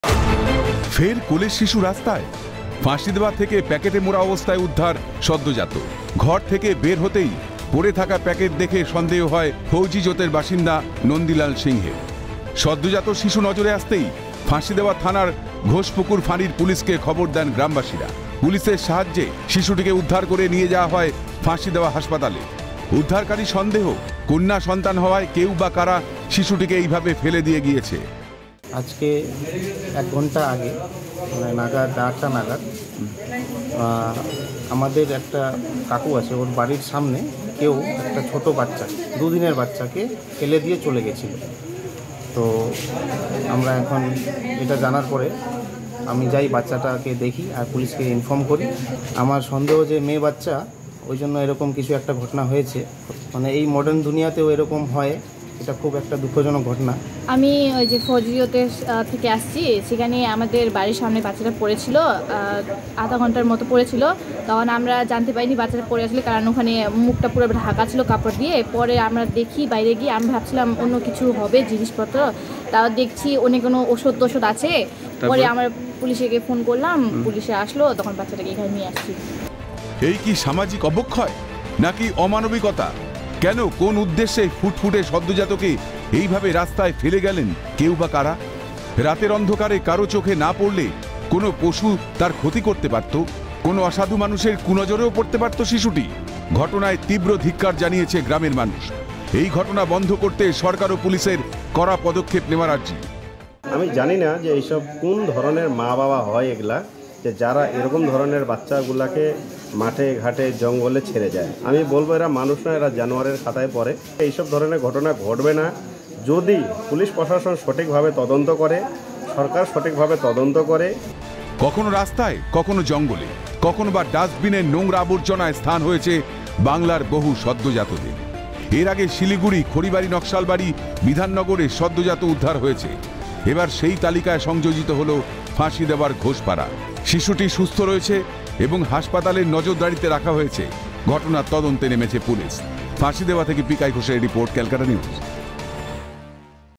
প্যাকেটে মোড়া সদ্যোজাত শিশু উদ্ধার | ক্যালকাটা নিউজ
প্যাকেটে মোড়া সদ্যোজাত শিশু উদ্ধার

ফাঁসিদেওয়ায় সদ্যোজাত কন্যা সন্তান উদ্ধার ঘিরে চাঞ্চল্য। ঘোষপুকুর ফাঁড়ির পুলিস উদ্ধার করে শিশুটিকে ফাঁসিদেওয়া হাসপাতালে নিয়ে যায়। বাবা মার হদিস মেলেন। হাসপাতালেই নজরদারিতে রয়েছে শিশুটি।